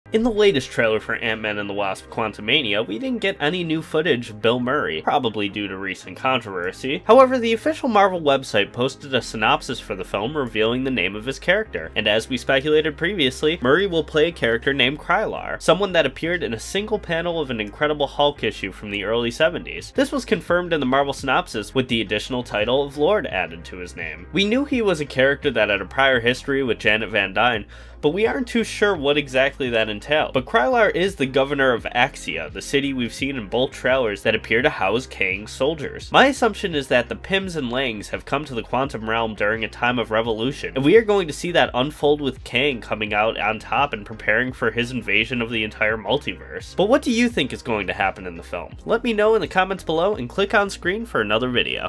The Mystery behind Ant-Man's SECRET VILLAIN
Bill Murray's MCU debut could also point to a villainous turn for the character Krylar and could lead to big things in the upcoming Avengers duology.